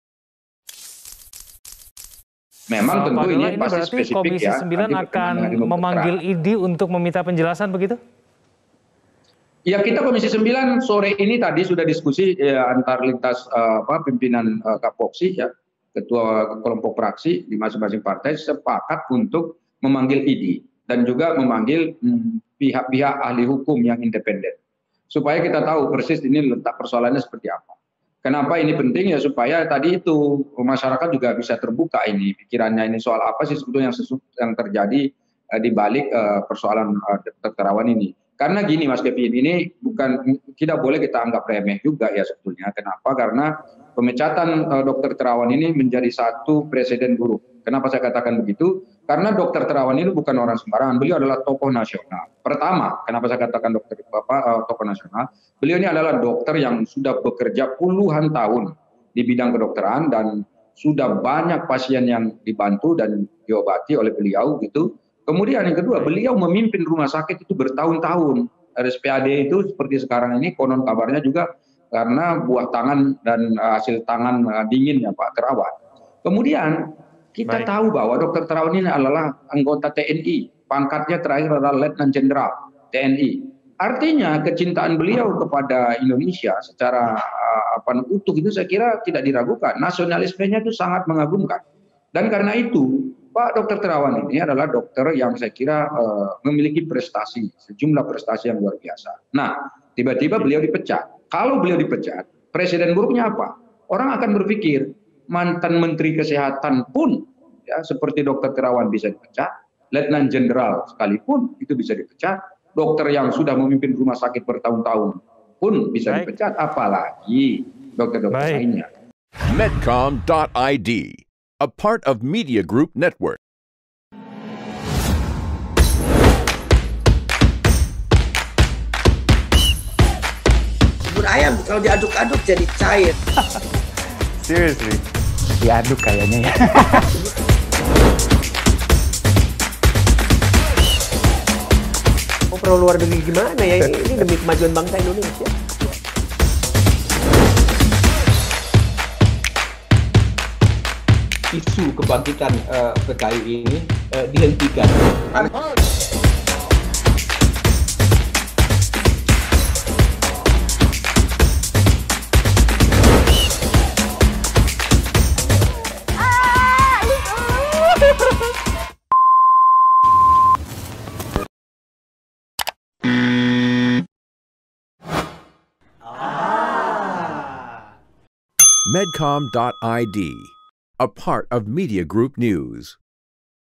Pemecatan Dokter Terawan dari IDI Dinilai Bisa Jadi Preseden Buruk
Anggota Komisi IX DPR Saleh Partaonan Daulay menyayangkan pemecatan dokter Terawan Agus Putranto dari keanggotaan Ikatan Dokter Indonesia (IDI). Menurut Daulay, banyak prestasi yang sudah ditorehkan Terawan. Sehingga, Daulay menilai pemecatan dokter Terawan bisa menjadi preseden buruk.